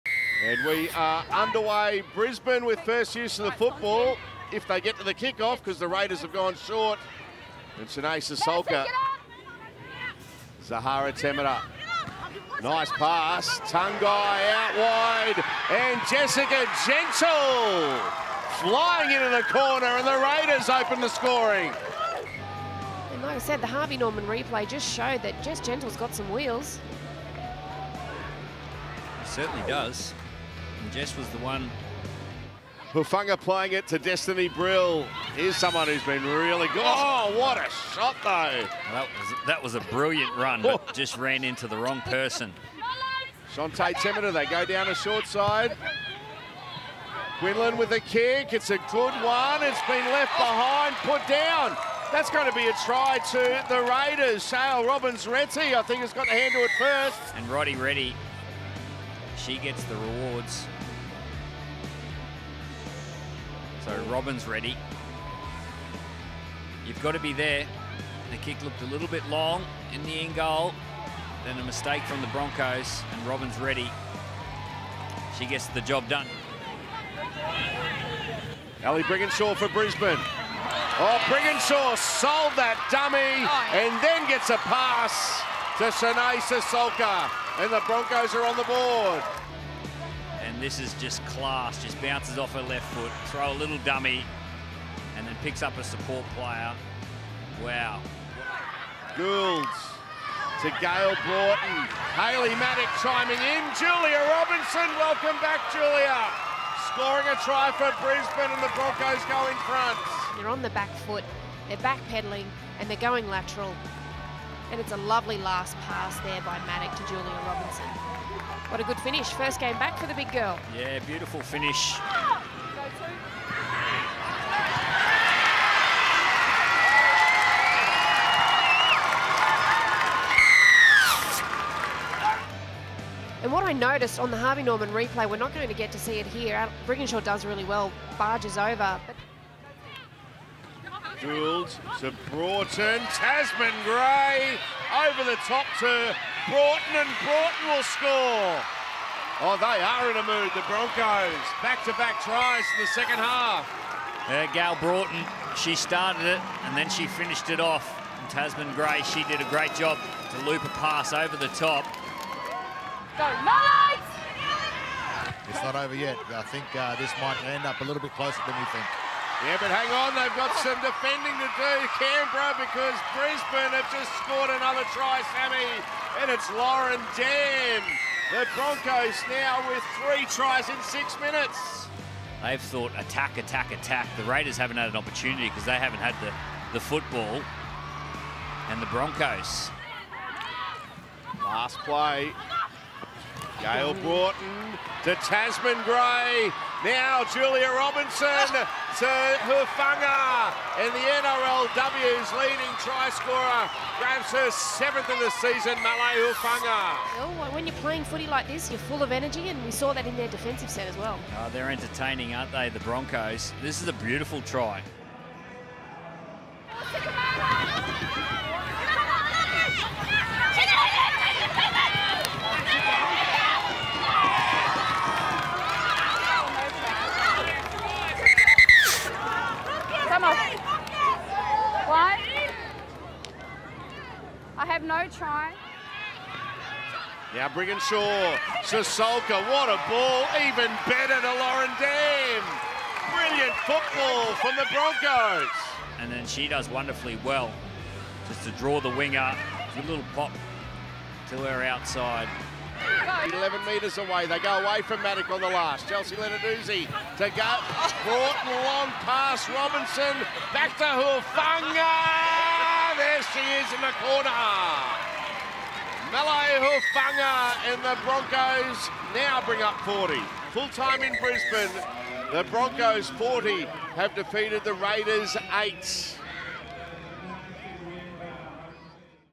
NRLW 2023 | Broncos v Raiders | Match Highlights
The Brisbane Broncos host the Canberra Raiders during the 2023 Telstra Women's Premiership